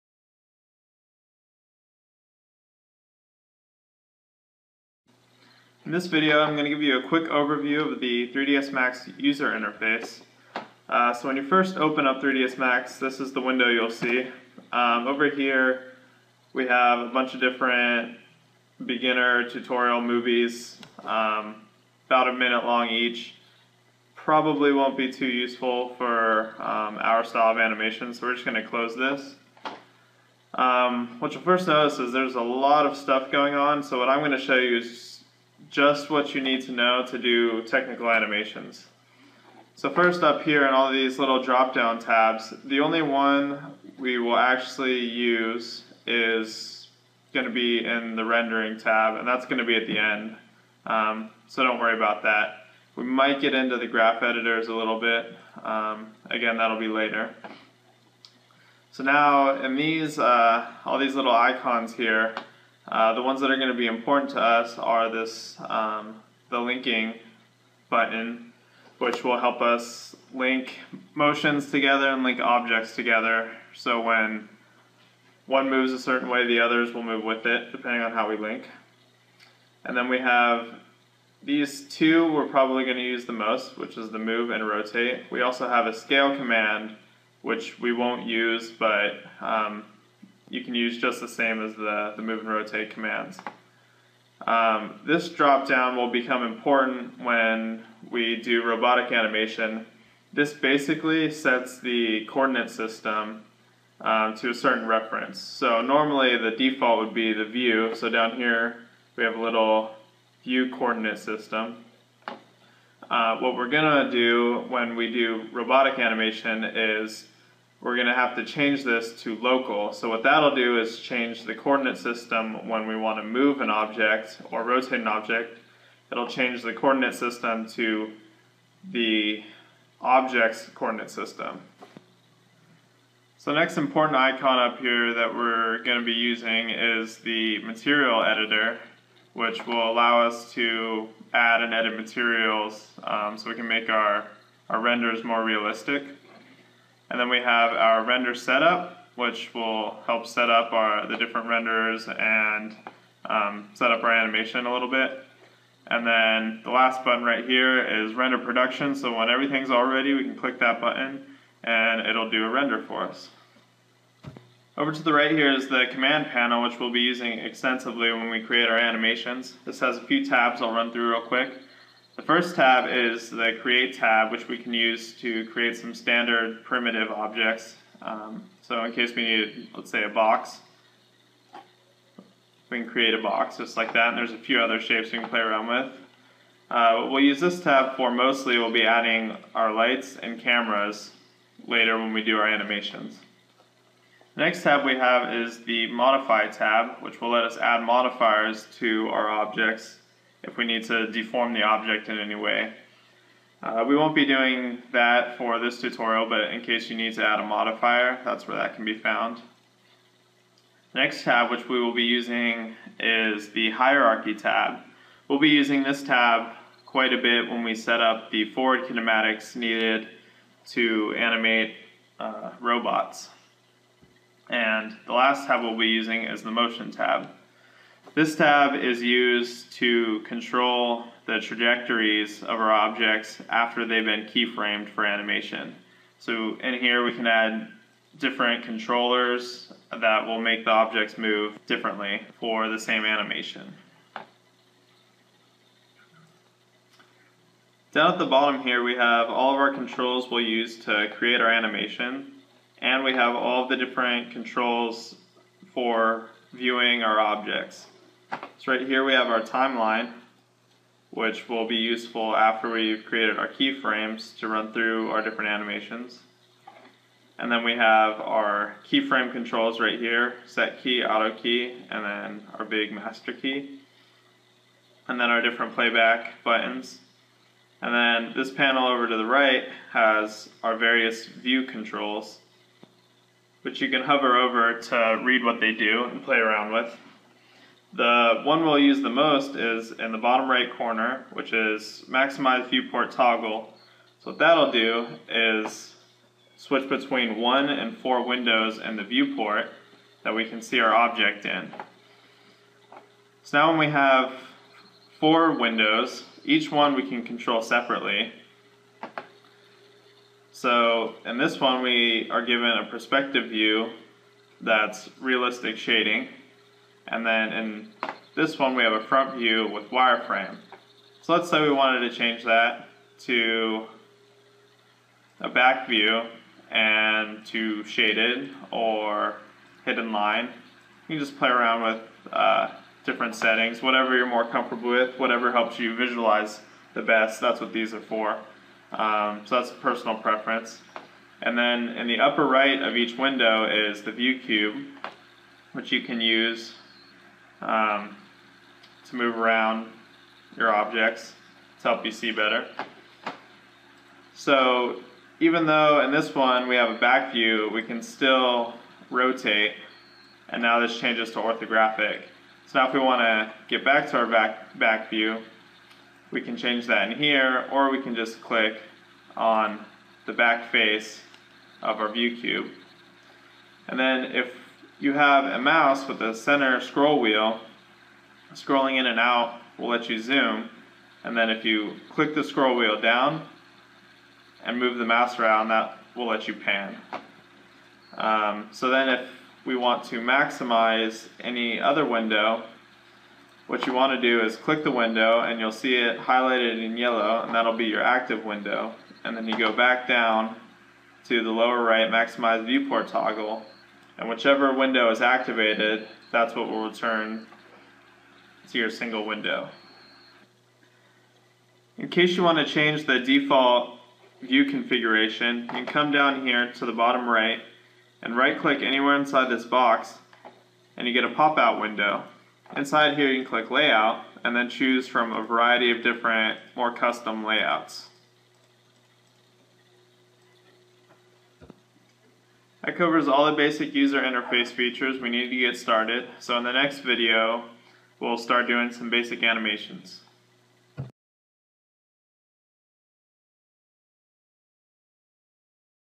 2 - 3ds Max User Interface Overview
In this video, we walk through the 3ds Max Design user interface as it pertains to making technical factory animations.

To get all of the files you need to complete this series, follow this link:

 Open the folder labeled "3ds Max workflow tutorial - Read Me" for information on getting everything set up.